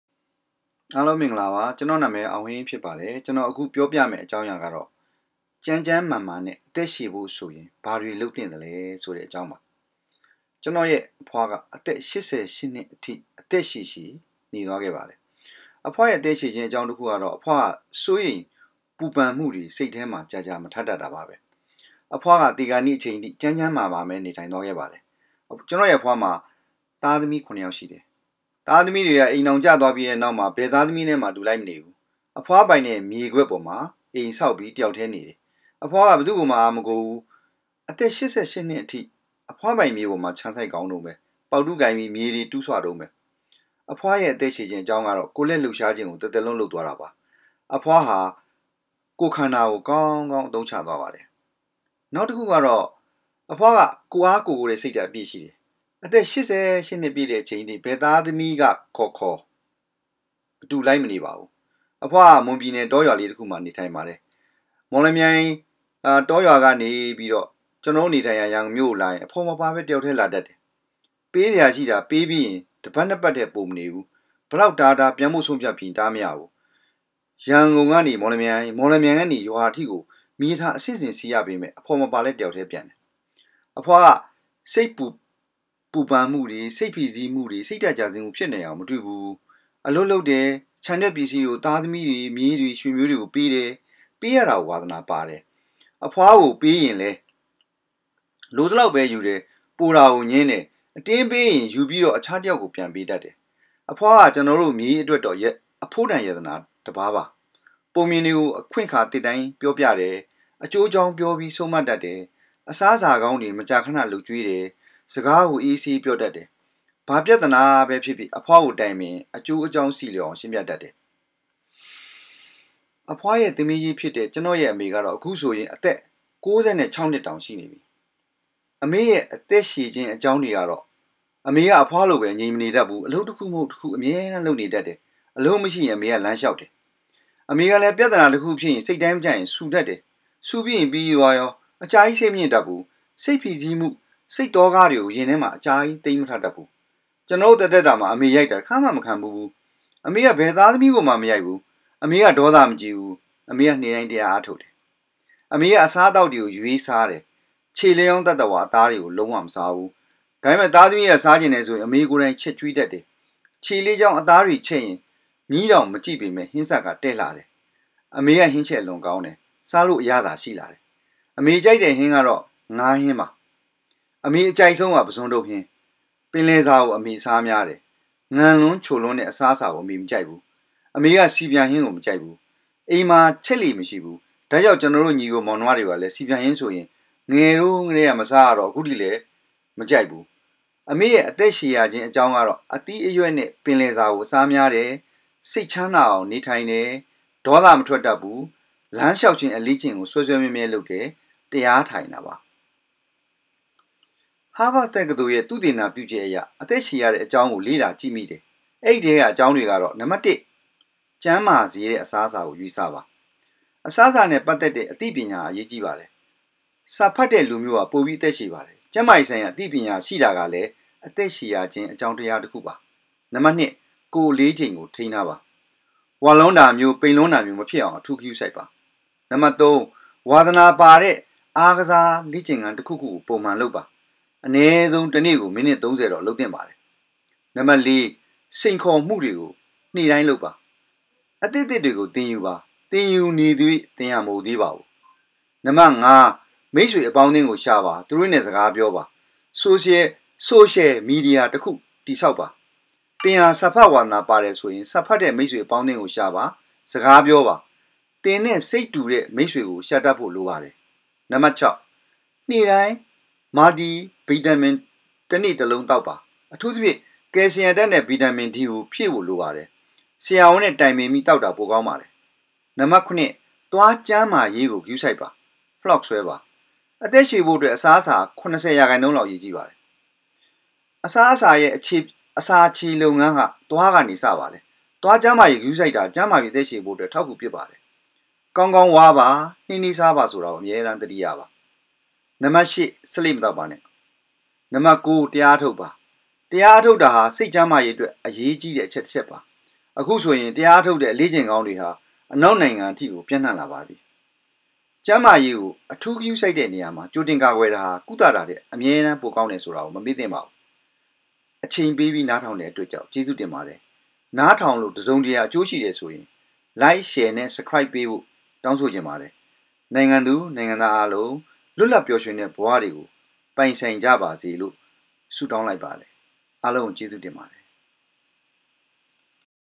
”က်န္းက်န္းမာမာနဲ႕ အသက္ရွည္ဖို႕ ဆိုရင္ ဘာေတြလုပ္သင့္သလဲ...”
က်န္းမာေရးကို အထူးဂရုစိုက္တဲ့ေနရာမွာ
ႀကိဳတင္ကာကြယ္တာဟာ ကုသတာထက္
အၿမဲတမ္းပိုေကာင္းတယ္ဆိုတာကို
မေမ့သင့္ပါဘူး...။

xxxxx

ဟားဗက္ ေဆးတကၠသိုလ္ရဲ႕ သုေတသနျပဳခ်က္အရ
အသက္ရွည္ရတဲ့အေၾကာင္းကို ေတြကေတာ့...

အစားအစာနဲ႕ ပတ္သက္တဲ့ အသိပညာဟာ အေရးႀကီးပါတယ္...။
စာဖတ္တဲ့လူမ်ိဳးဟာ ပိုၿပီး အသက္ရွည္ပါတယ္...။
က်န္းမာေရးဆိုင္ရာ အသိပညာရွိတာကလည္း
အသက္ရွည္ရတဲ့အေၾကာင္းတရားတစ္ခုျဖစ္တယ္…

Xxxxx

ဝလြန္းတာမ်ိဳး၊ ပိန္လြန္းတာမ်ိဳး မျဖစ္ေအာင္ အထူးဂရုစိုက္ပါ...။

Xxxxx

အနည္းဆံုး တစ္ေန႕ကို မိနစ္ ၃၀- လုပ္သင့္ပါတယ္...။

Xxxxx

၄။   စိန္ေခၚမႈေတြကို ေန႕တိုင္း လုပ္ပါ...။
အသစ္အသစ္ေတြကို သင္ယူပါ...။

သင္ယူေလ့လာေနသေရြ႕ သင္ဟာ မအိုပါဘူး...။

Xxxxx

၅။  မိတ္ေဆြအေပါင္းအသင္းကို ရွာပါ...။ သူတို႕ နဲ႕ စကားေျပာပါ...။

ဆိုရွယ္ မီဒီယာ တစ္ခု တည္ေဆာက္ပါ...။

သင္က စာဖတ္ဝါသနာပါတယ္ဆိုရင္
စာဖတ္တဲ့ မိတ္ေဆြ အေပါင္းအသင္းကို ရွာပါ...။ စကားေျပာပါ...။

သင္နဲ႕ စိတ္တူတဲ့ မိတ္ေဆြကို ရွာတတ္ဖို႕ လိုပါတယ္...။

Xxxxx

၆။  ေန႕တိုင္း Multi- vitamin တစ္ေန႕ တစ္လံုးေသာက္ပါ...။

အထူးသျဖင့္ ကယ္ဆီယမ္ နဲ႕  ဗီတာမင္ ဒီကို ျဖည့္ဖို႕ လိုပါတယ္...။
ဆရာဝန္နဲ႕ တိုင္ပင္ၿပီး ေသာက္တာ ပိုေကာင္းပါတယ္...။

Xxxxx

၇။   သြားက်န္းမာေရးကို ဂရုစိုက္ပါ...။ ဖေလာ့ ဆြဲပါ...။

အသက္ရွည္ဖို႕အတြက္ အစားအစာဟာ ၇၀% ေလာက္
အေရးႀကီးပါတယ္...။အစားအစာရဲ႕ အစာေျချခင္းလုပ္ငန္းဟာ
သြားက စတင္ပါတယ္...။ သြားက်န္းမာေရးကို ဂရုစိုက္တာဟာ
က်န္းမာၿပီး အသက္ရွင္ဖို႕ အေထာက္အကူျပဳပါတယ္...။
ေကာင္းေကာင္း ဝါးပါ...။
ေႏွးေႏွးစားပါ...ဆိုတာကို အၿမဲတမ္း သတိရပါ...။

Xxxxx

Xxxxx